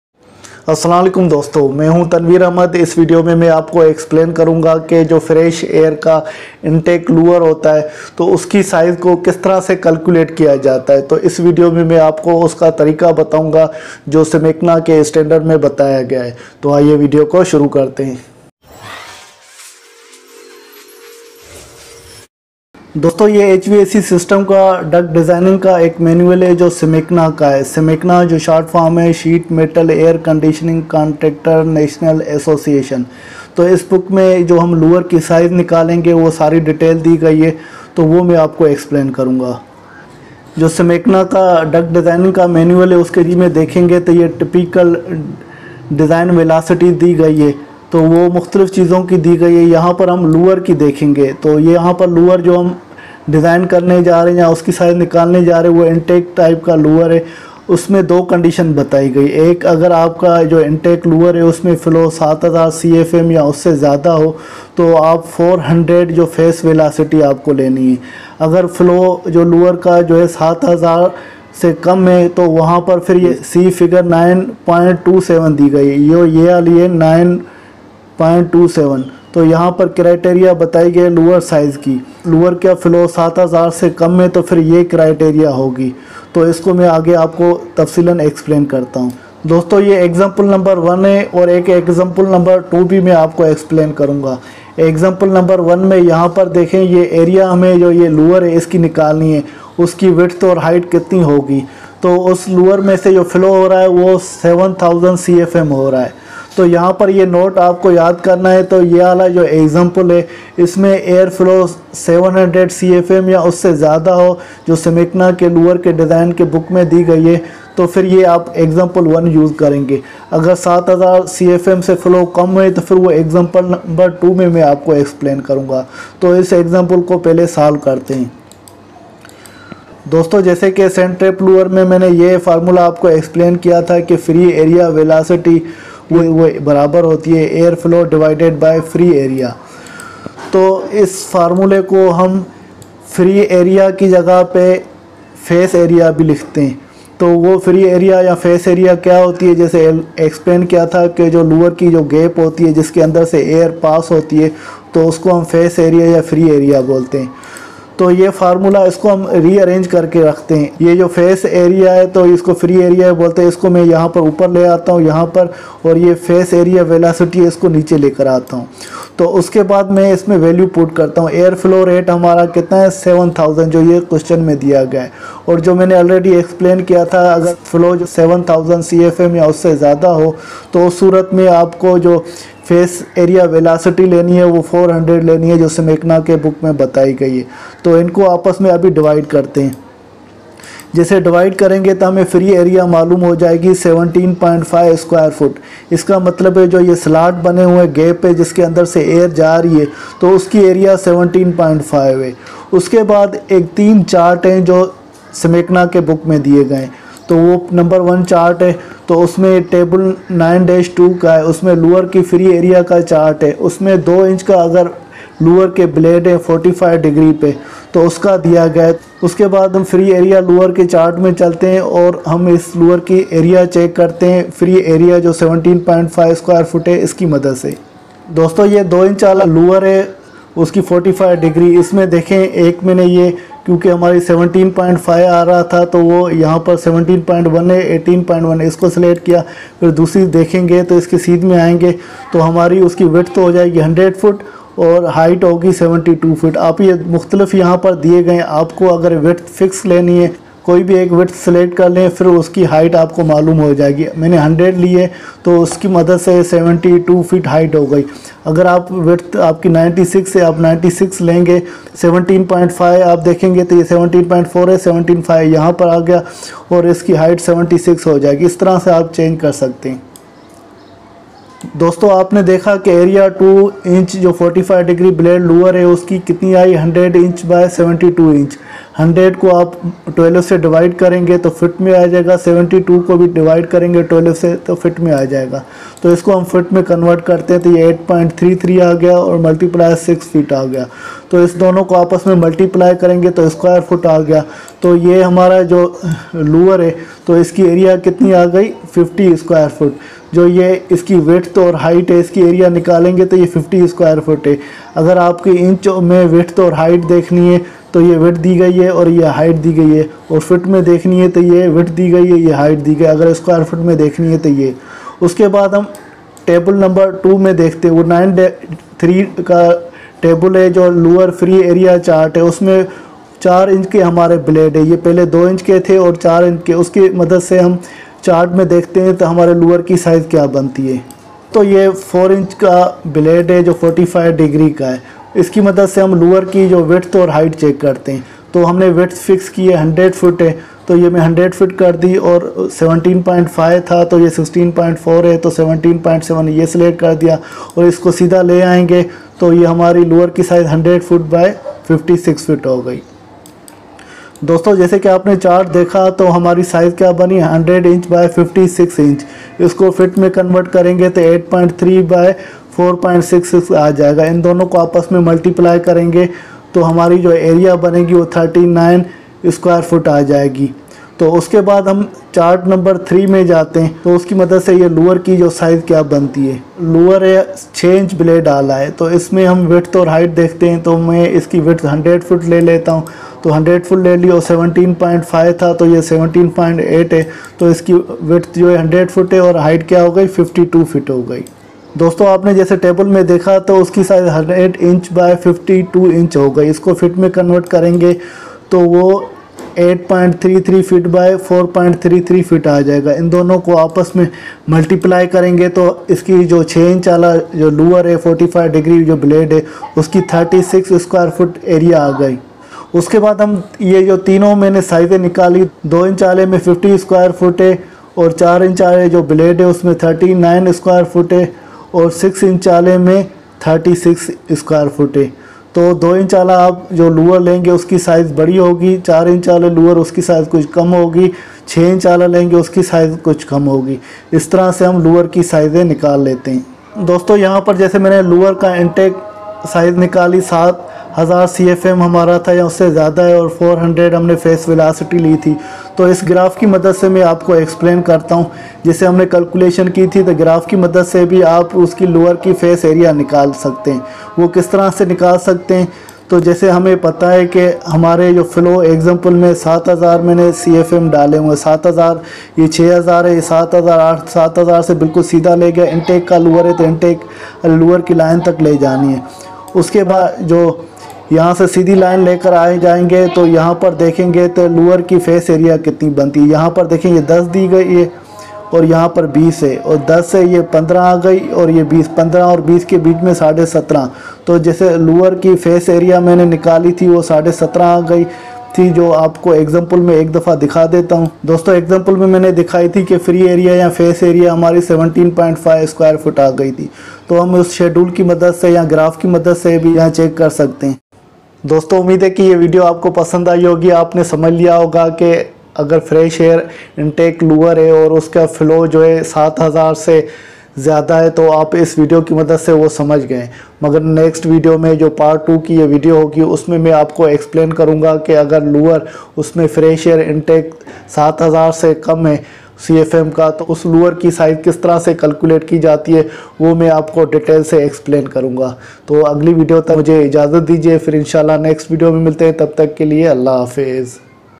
How to Size Intake Air Louver | HVAC Louver Part 1 in Urdu/Hindi
This is the Air devices tutorial video about "How to size intake air louver flow greater than 7000 cfm or how to calculate size of louver or fresh air louver sizing" in Urdu/Hindi. It is also important for HVAC/MEP technical staff. Here sharing my experience and knowledge that will be helpful for learning & growing for Engineers & Technical persons.